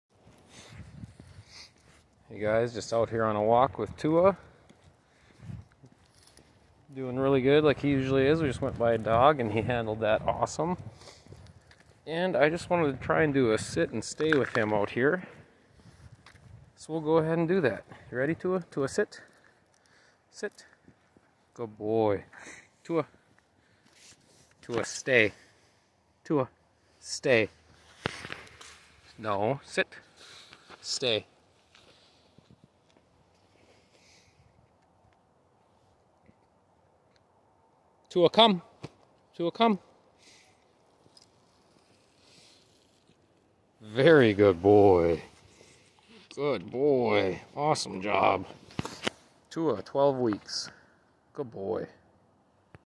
Bullmastiff 12 weeks old Sit. Stay. Come!
Out for a walk with Tua the bullmastiff and wanted to try a sit, stay, come in an unfamiliar environment

Weekly videos will be posted to show not only his physical growth, but his behavioral changes, obedience level changes, and our overall experience with the Bullmastiff breed.